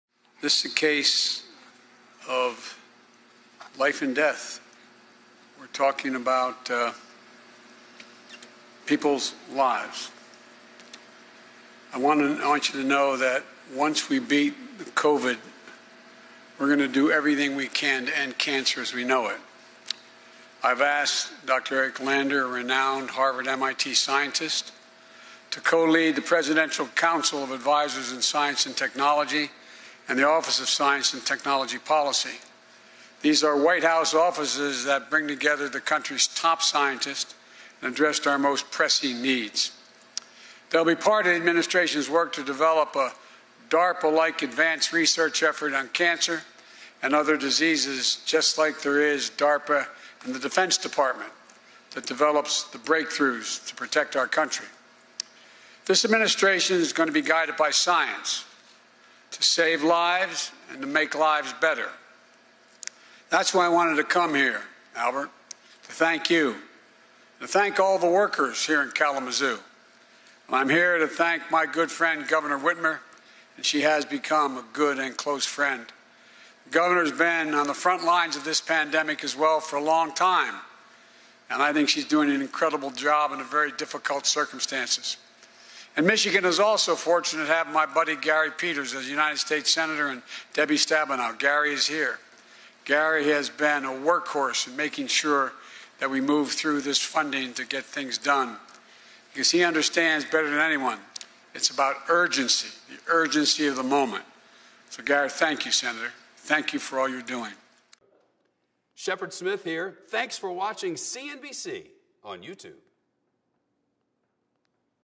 President Joe Biden: After we beat Covid, we're going to 'end cancer as we know it'
President Biden speaks at the Pfizer manufacturing site about the coronavirus pandemic and the efforts made during the vaccine rollout, as well as his plans to defeat cancer.

President Joe Biden said Friday that once the coronavirus pandemic has been defeated, his administration will target another deadly disease: Cancer.

“I want you to know that, once we beat Covid, we’re going to do everything we can to end cancer as we know it,” Biden said in a speech after touring Pfizer’s massive coronavirus vaccine manufacturing site in Kalamazoo, Michigan.

Cancer is the second-leading cause of death in the U.S., according to the National Center for Health Statistics, with nearly 600,000 people dying of cancer in 2019. Almost 1.9 million new cancer cases will be diagnosed in the U.S. in 2021, American Cancer Society researchers estimate.

One of Biden’s sons, Beau Biden, died at age 46 from an aggressive form of brain cancer.

Biden said that two White House offices, the President’s Council of Advisors on Science and Technology and the Office of Science and Technology Policy, will be involved in developing an “advanced research effort on cancer and other diseases.”

Dr. Eric Lander, the director of the Broad Institute of MIT and Harvard University, will co-lead both offices, Biden said.

The president likened the initiative to DARPA, the Pentagon agency tasked with testing new technologies.

As a presidential candidate, Biden had proposed the creation of such an agency as part of the “Made in America” plank of his platform. His campaign website called it the Advanced Research Projects Agency for Health, or ARPA-H.

Then-candidate Biden would reportedly bring the proposal up frequently during private campaign fundraisers, though he rarely talked about it at public events.

Biden’s forward-looking announcement appeared intended to send the message that his administration has tightened its grip on the pandemic.

That message was underscored by the location in which he chose to deliver it: a 1,300-acre vaccine manufacturing site where millions of doses of Pfizer’s Covid vaccine are being produced, packaged, frozen and shipped out.

“We’re now at a point where we’ve seen the average daily number of people vaccinated nearly double from the week before I took office, to about 1.7 million average per day getting a shot,” Biden said, adding, “We’re on track to surpass my commitment” to administer 100 million shots in his first 100 days as president.

But “despite the progress, we’re still in the teeth of a pandemic,” Biden cautioned.

He noted that new strains of the virus are emerging, and that the U.S. is poised to soon surpass the grim milestone of 500,000 deaths due to Covid.

“If there’s one message to cut through to everyone in this country it’s this: The vaccines are safe. Please, for yourself, your family, your community, this country, take the vaccine when it’s your turn and available,” Biden said.

Biden urged Americans to continue to take precautions for their health and safety, including hand-washing, social distancing and wearing masks.

“Look, I know it’s inconvenient but you’re making a commitment when you do it,” Biden said. “Everyone needs to do their part for themselves, their loved ones and, yes, their country. It’s a patriotic duty.”